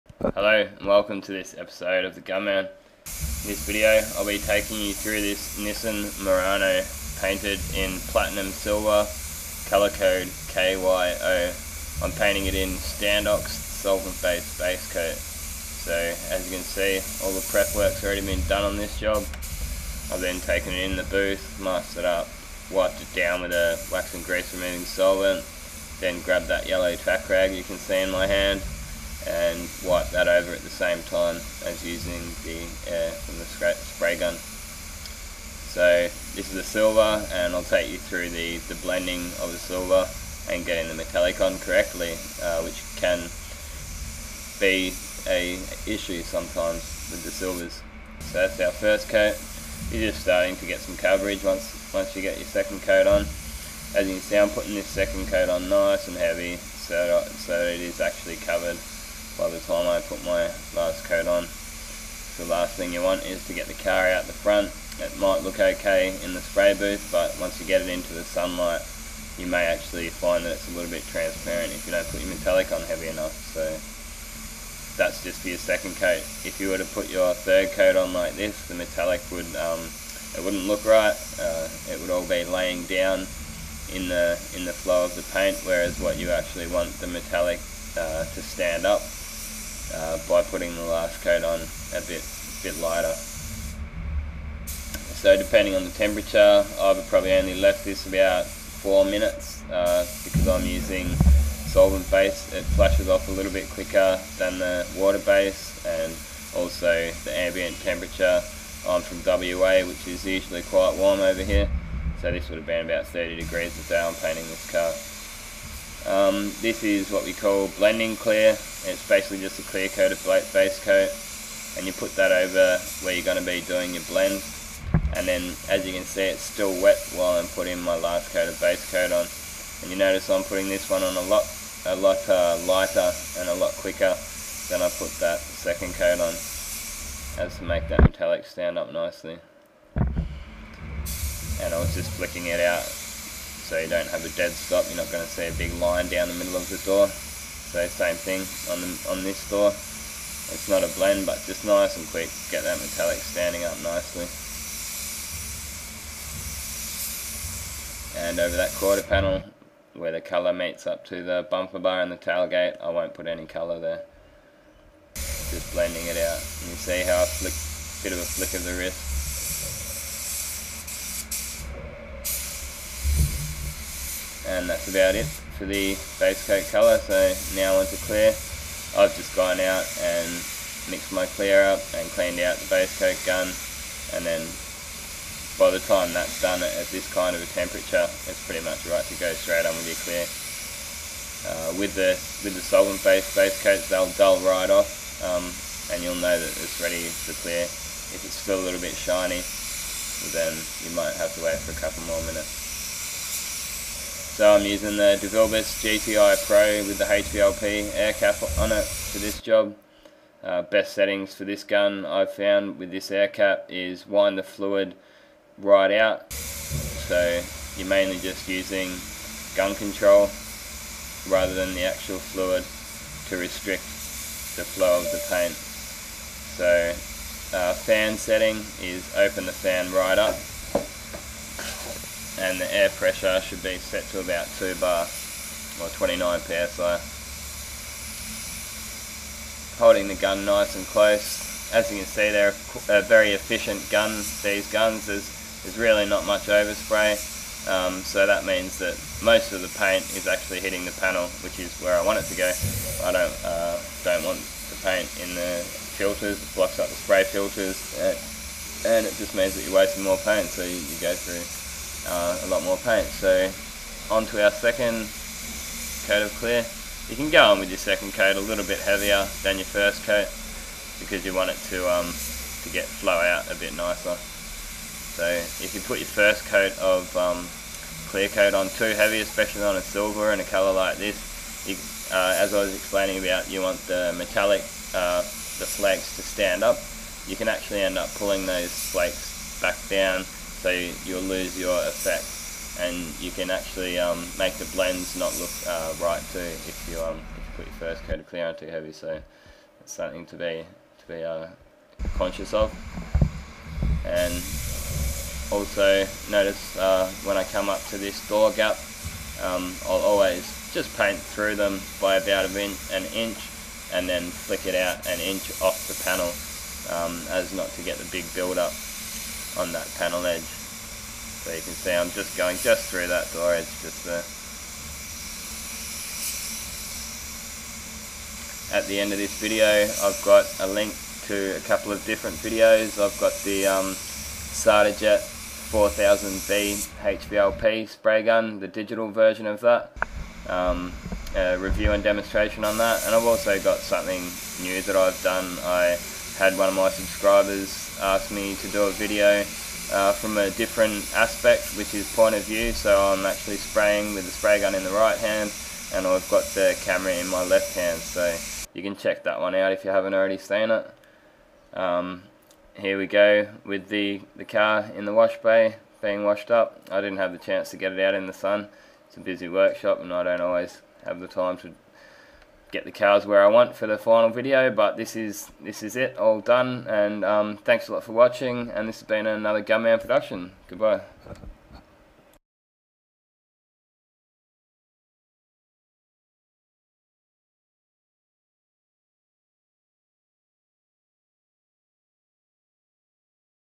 Blending Metallic Paint & 2K Clear Coat
In this video I will take you through the steps of Spray Painting this Nissan Murano in Platinum Silver code KYO, in Standox Solvent Base and Crystal Clear Pro HS Clear with a DeVilbiss GTI Pro HVLP Spray Gun.  Learn how to Spray Paint like a Pro by following this step by step guide to Automotive Spray Painting.